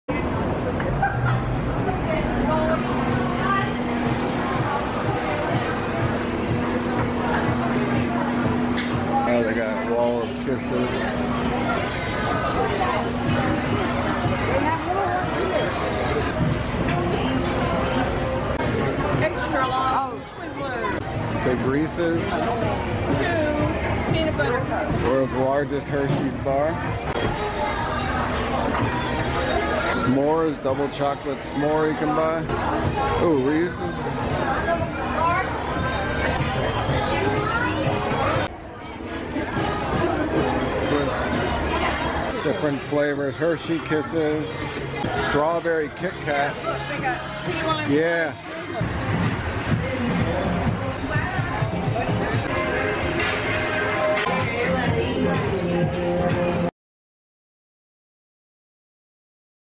We Visited The Hershey Store in Times Square
While in New York City, we visited the Hershey Store in Times Square